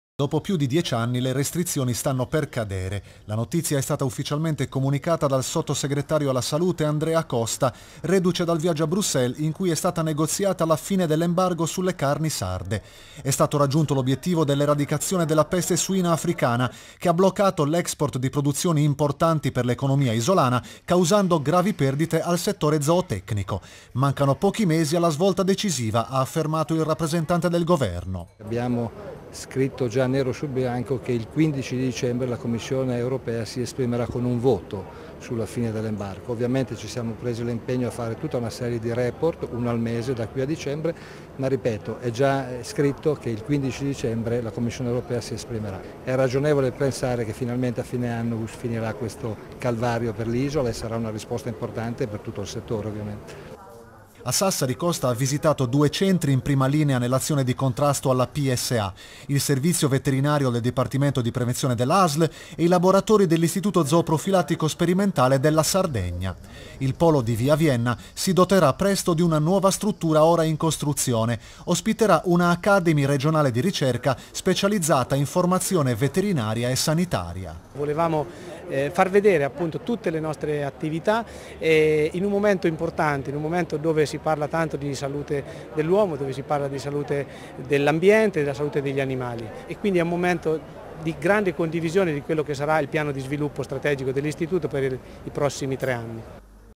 Peste suina, Costa: "Ragionevole pensare che a fine anno termineranno le restrizioni"
Peste suina - Andrea Costa, sottosegretario di Stato al Ministero della salute: "Il 15 dicembre la commissione europea si esprimerà con un voto sulla fine dell'embargo. Abbiamo preso l'impegno a realizzare una serie di report ed è ragionevole pensare che a fine anno termineranno le restrizioni. Sarà una risposta importante per tutto il settore."
Il Direttore generale dell'Istituto zooprofilattico sperimentale della Sardegna, Giovanni Filippini: "Volevamo far vedere tutte le nostre attività in un momento importante nel quale si parla di salute dell'uomo, dell'ambiente e degli animali. É un momento di grande condivisione di quello che sarà il piano di sviluppo dell'Istituto zooprofilattico sperimentale della Sardegna per i prossimi tre anni."

Tgr Sardegna, 21 luglio 2022